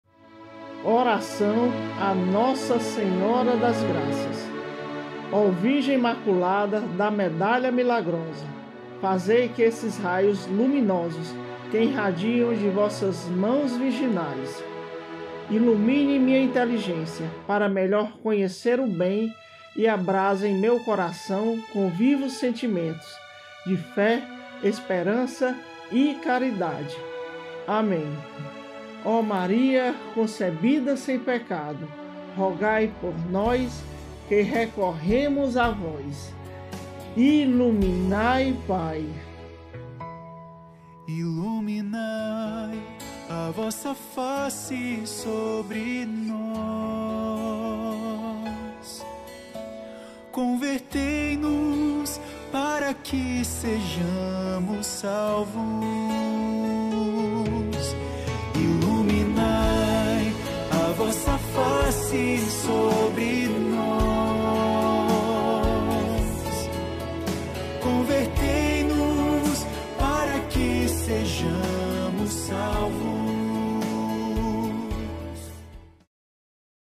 30/10/2024 ORAÇÃO a NOSSA SENHORA das GRAÇAS que nos irradiam raios luminosos de amor e paz
ORAÇÃO a NOSSA SENHORA DAS GRAÇAS
O Virgem Imaculada da Medalha Milagrosa,
fazei que esses raios luminosos que irradiam
de vossas mãos virginais, iluminem minha inteligência
para melhor conhecer o bem, e abrazem meu coração com
vivos sentimentos de fé, esperança e caridade.
Amém.

Ó Maria concebida sem pecado,
rogai por nós que recorremos a Vós. (3 vezes)

Avé Maria, cheia de graça, o Senhor é convosco,
bendita sois vós entre as mulheres
e bendito é o fruto do vosso ventre, Jesus.
Santa Maria, Mãe de Deus, rogai por nós pecadores,
agora e na hora da nossa morte. Ámen

Dia 27 de Novembro é o dia de Nossa Senhora das Graças
Nossa Senhora das Graças. rogai por nós que recorremos a vós!
Nossa Senhora das Graças, rogai por nós!
Jesus, eu confio em Vós!

📢  EVANGELIZE CONOSCO!

 Iluminai PAI

 🙏 Iluminai PAI 🙏

Dia 27 de Novembro Dia de Nossa Senhora das Graças
Que Deus derrame muitas graças para toda sua família! Amém!